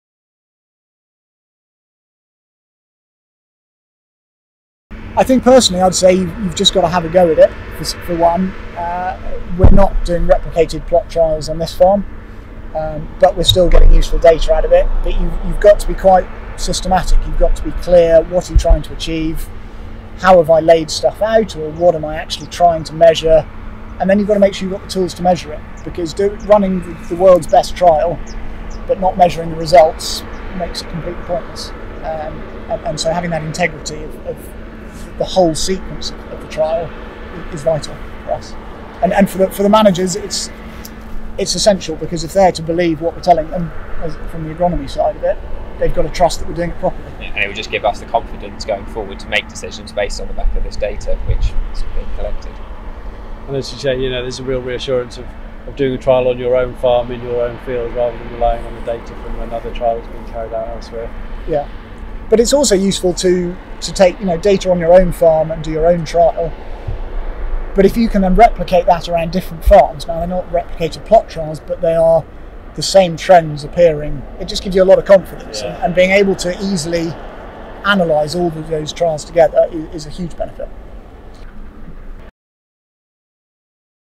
What advice would you give to farmers looking to develop trials on their own farm?
Measurement is the key to a successful trial. That's the main lesson to take from Olly Pemberton as he shares how FieldView data can help optimise data gathering.